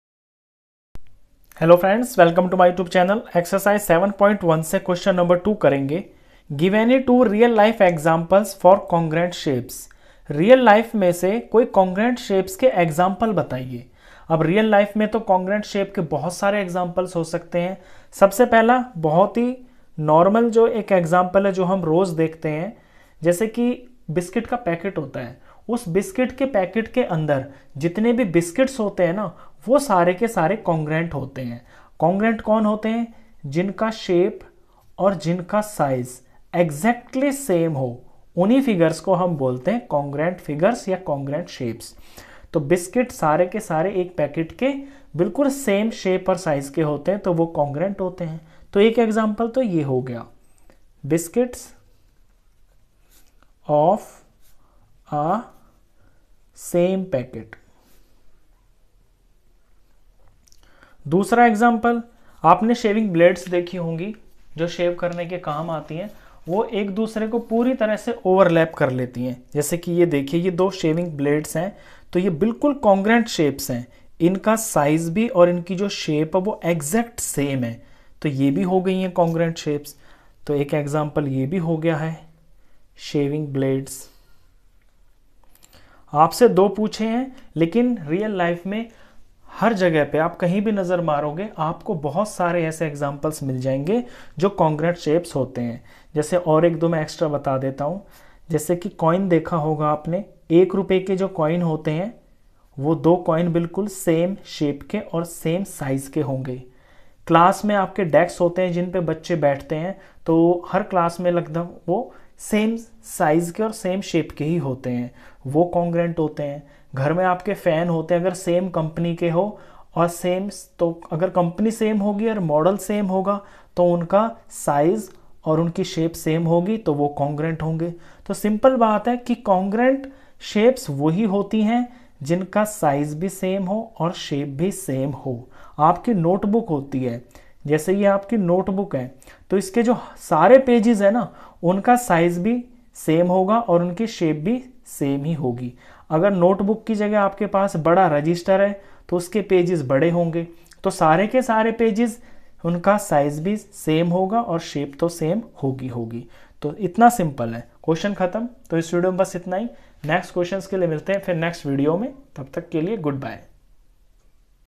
Q.2, Ex.7.1 Chapter:7 Congruence Of Triangles | Ncert Maths Class 7 | Cbse
In this video I taught Q.2 of exercise:7.1 from Chapter:7 Congruence Of Triangles | Ncert Maths Class 7 | Cbse Board.